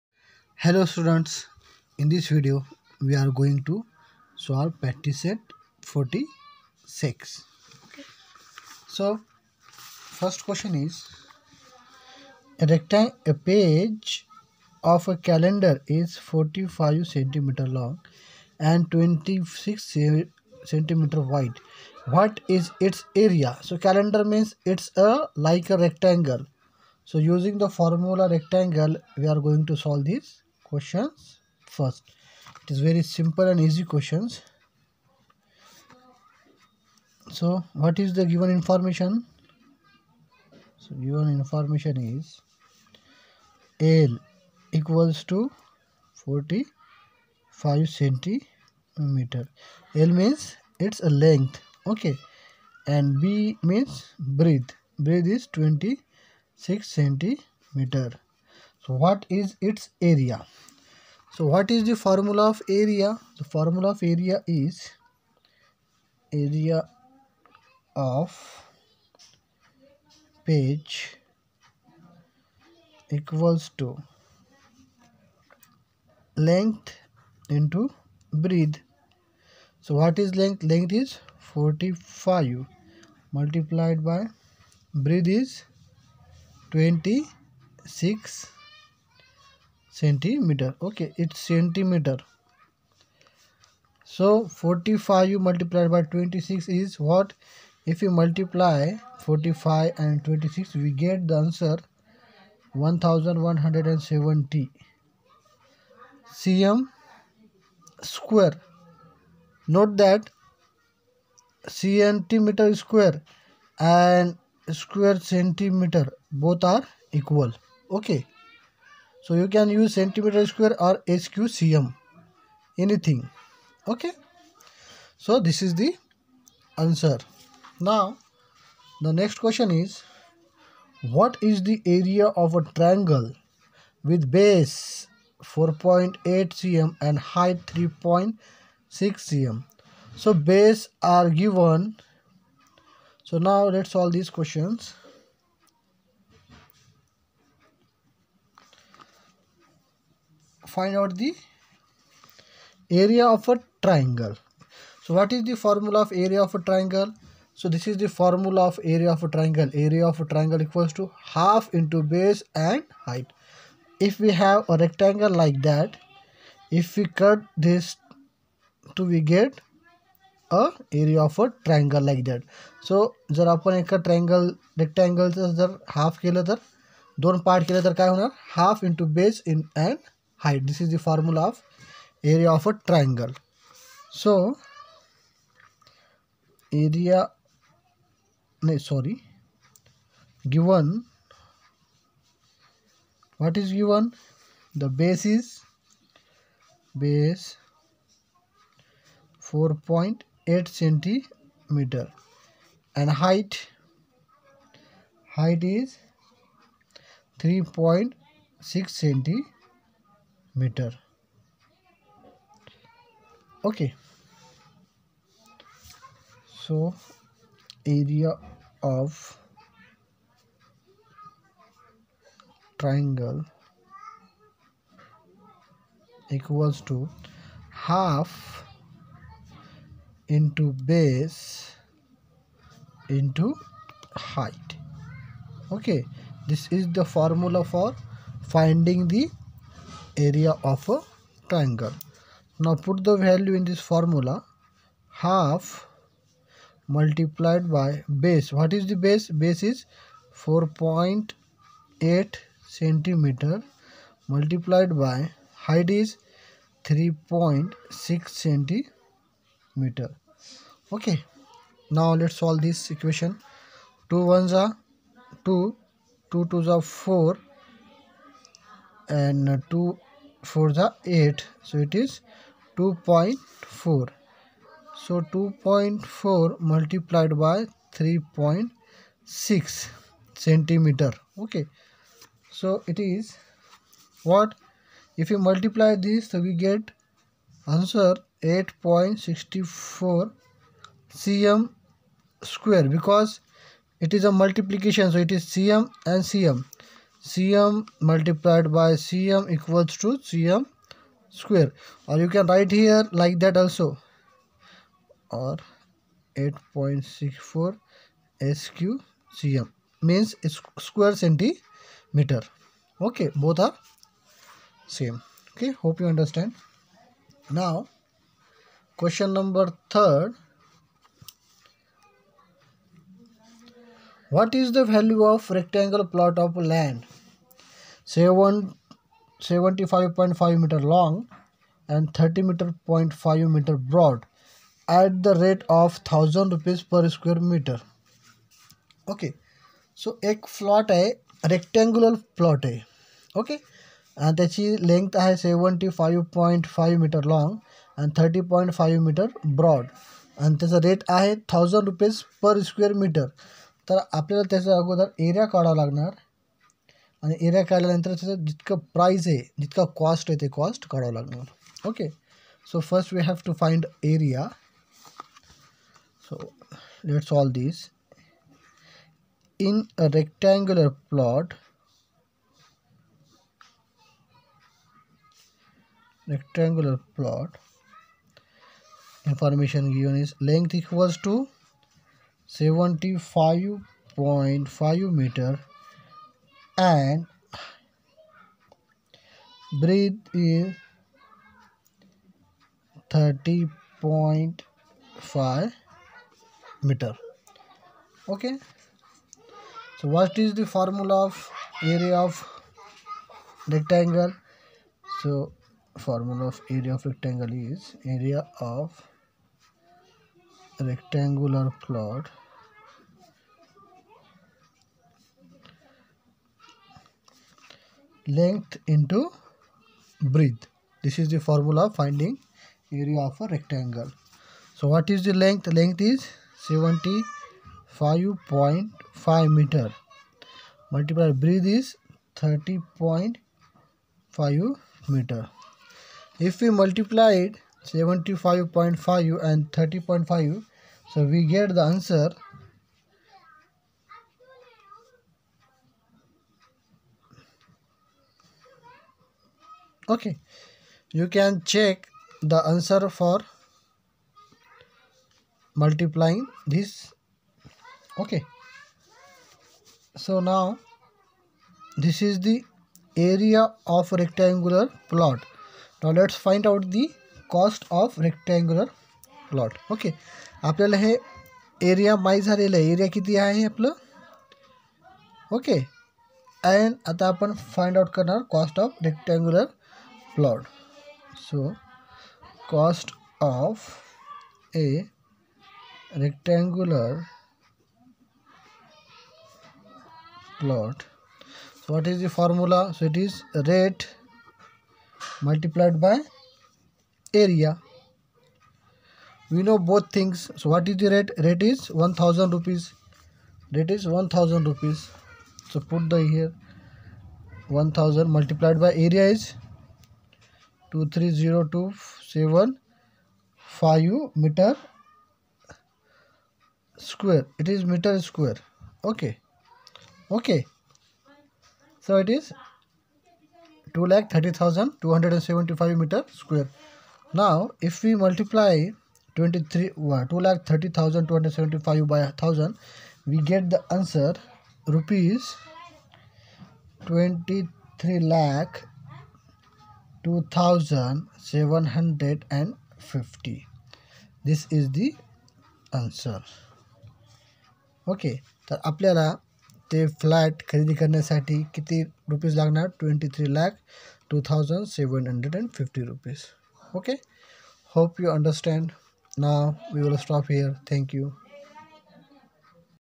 Practice set 46 (1) class 7
Hello student in this we are solving practice set 46 question no 1,2 and 3 class 7th state board of maharashtra